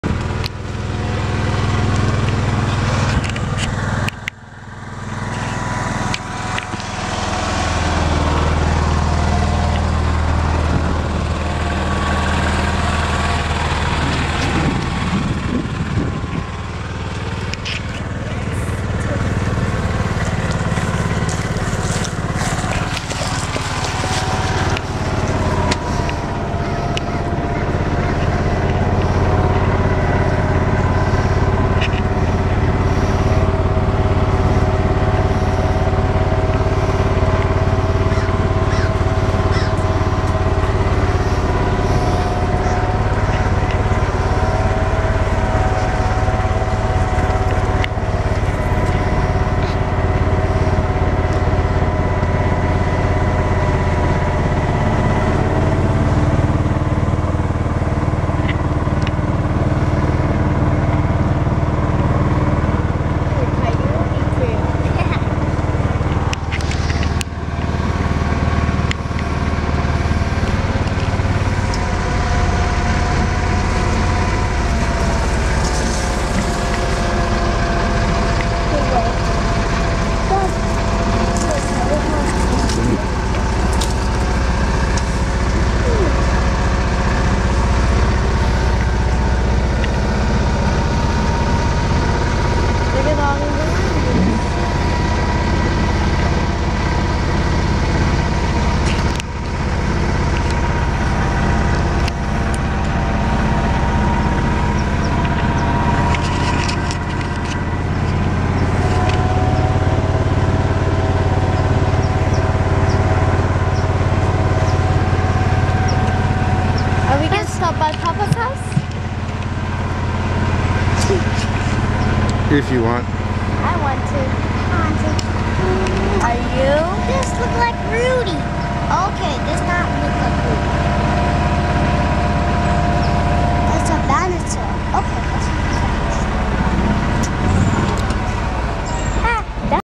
MONIQUE DRIVES THROUGH THE BUSHES
Monique Drives through the upper section of THE RANCH. Here she takes it nice ans slow. Erika hangs on to her Angry Bird.